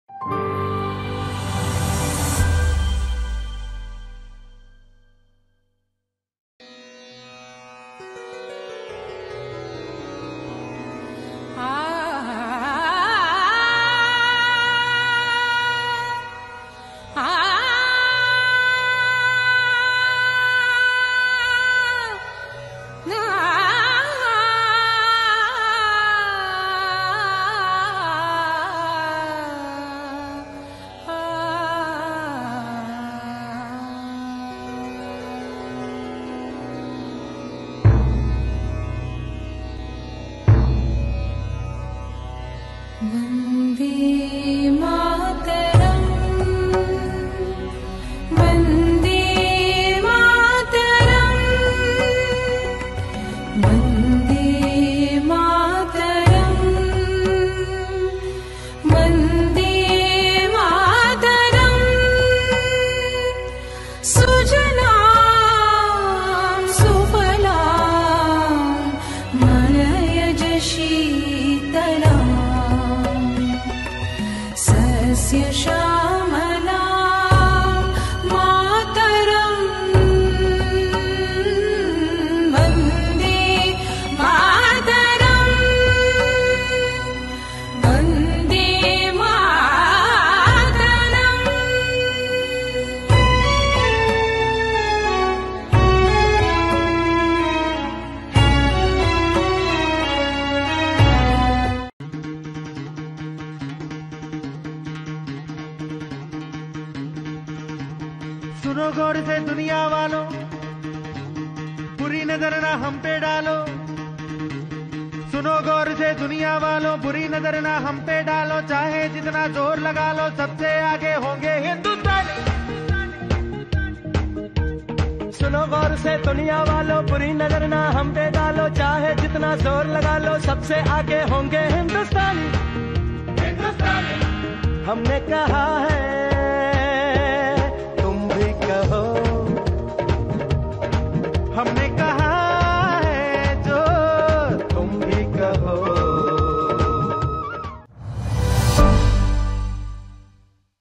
Diaspora celebrates Indian values and valour on Independence Day
Shades of diaspora celebrations in the UK for Indian Independence Day 2022.